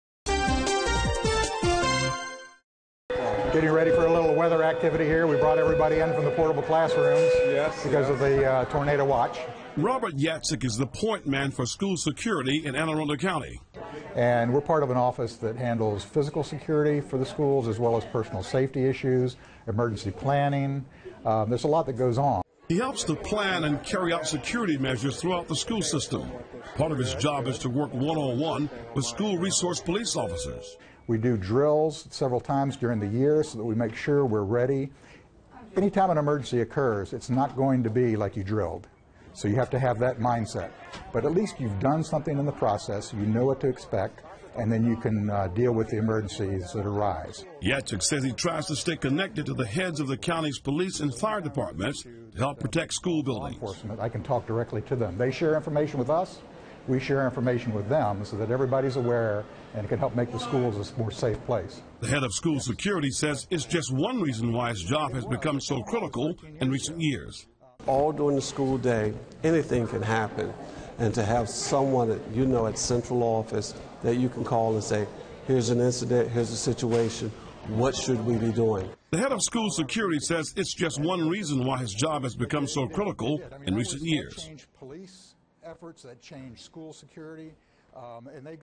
Anne Arundel County officials stress school safety
Anne Arundel County created its office of school security almost 10 years ago. Administrators said it's a position that's gone a long way toward helping to keep students safe.